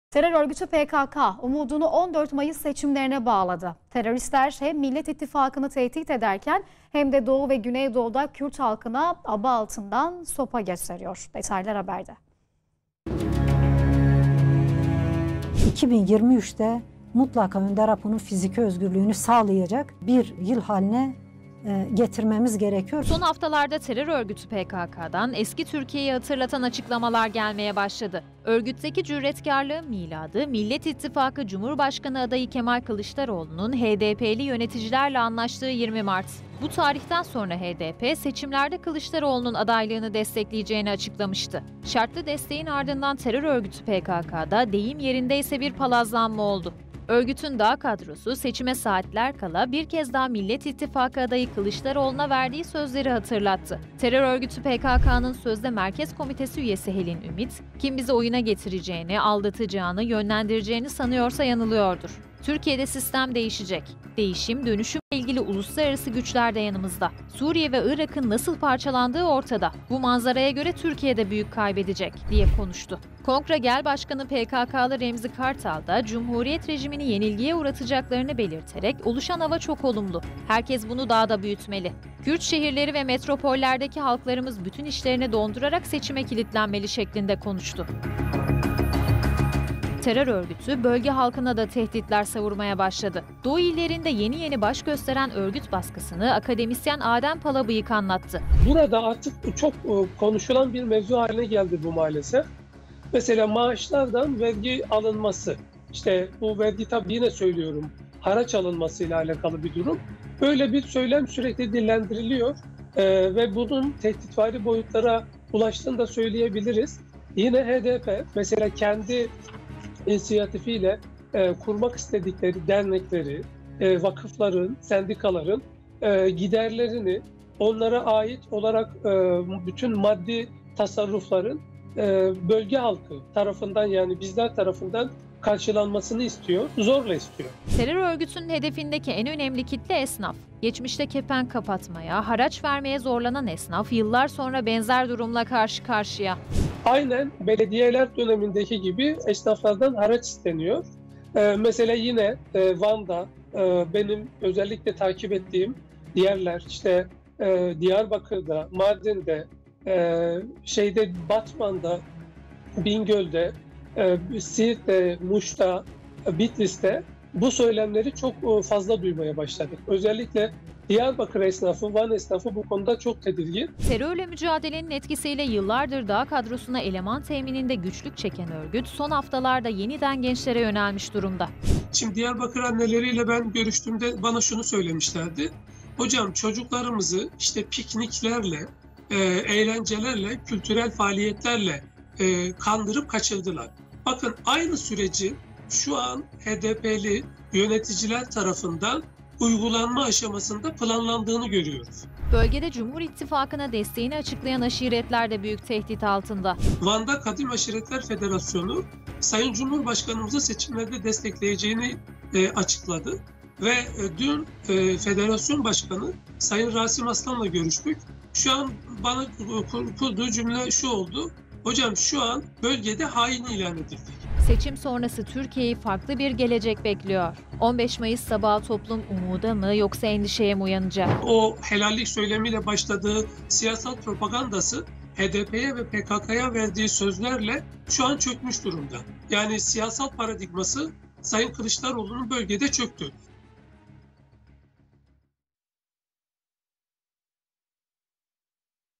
Kılıçdaroğlu-HDP anlaşması sonrası PKK yeniden hareketlendi
Terör örgütü PKK umudunu 14 Mayıs seçimlerine bağladı. Teröristler hem millet ittifakını tehdit ederken hem de Doğu ve Güney Doğu'da Kürt halkına sopa gösteriyor.